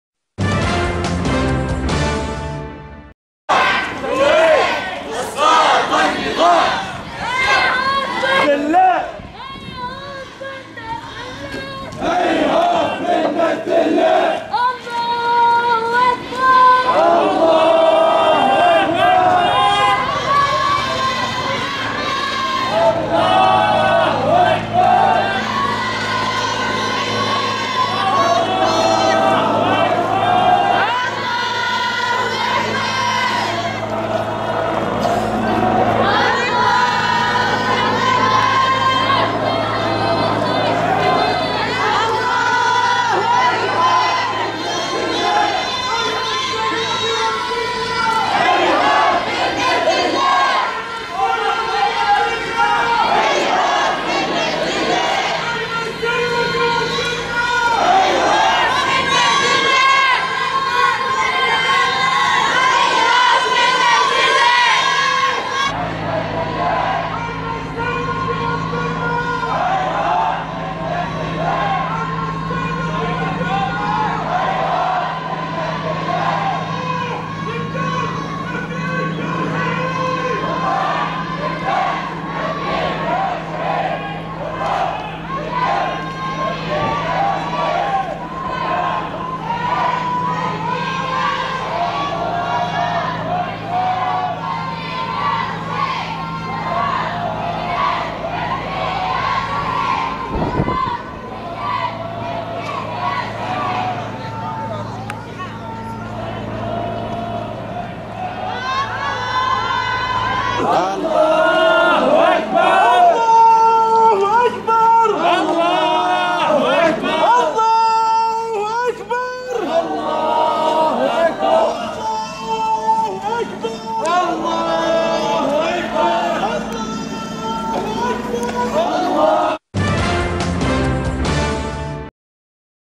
تظاهرة حاشدة بالنويدرات ضمن الجولة الثانية في اليوم الأول من عصيان العزة 13/2/2014 Bahrain
حشد من المواطنين يشاركون في مسيرة خرجت عصر اليوم الخميس (13 فبراير/شباط 2014) بمنطقة النويدرات جنوب العاصمة المنامة، المسيرة جاءت ضمن فعاليات الجولة الثانية من العصيان المدني الشامل "عصيان العزة" تزامنا مع الذكرى الثالثة للثورة في البحرين، المشاركون هتافات تطالب بإسقاط النظام الملكي الحاكم في البحرين قبيل أن تقمعهم قوات المرتزقة التابعة للنظام الخليفي.